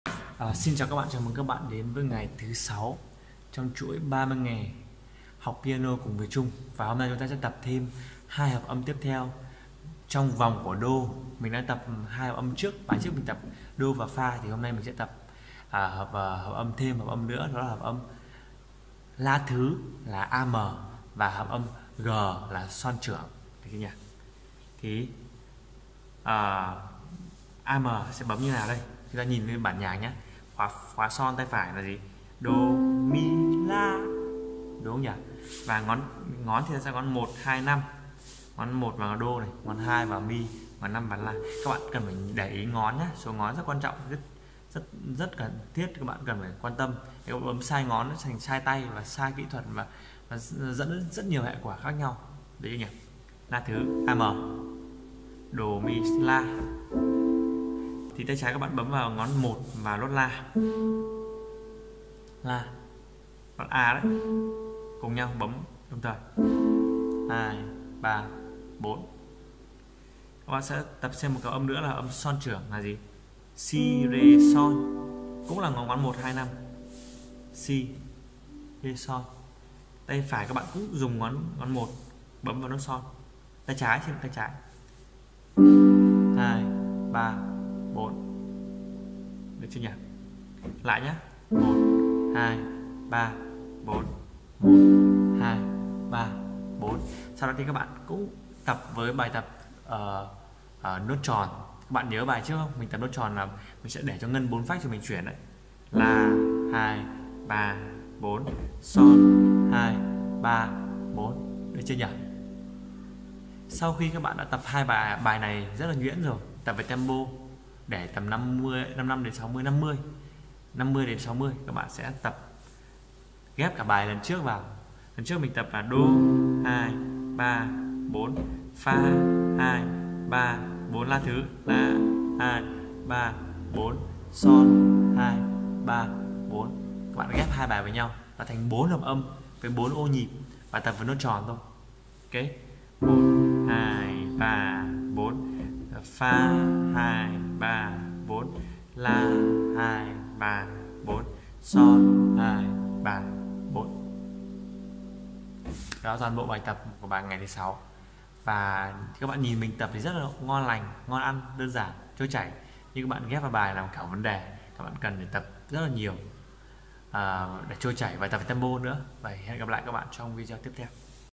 [Day 6] Bài Tập Trường Độ Tone C (P2) | 30 Ngày Học Piano Cùng Trung
Chào mừng các bạn đến với series 30 ngày học piano cùng Trung. Đây là loạt bài chia sẻ lộ trình học piano cơ bản để giúp cho những bạn nào muốn học piano đệm hát có thể tham khảo. Chúc các bạn thành công.!

*Vietcombank: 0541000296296 NGUYEN VAN TRUNG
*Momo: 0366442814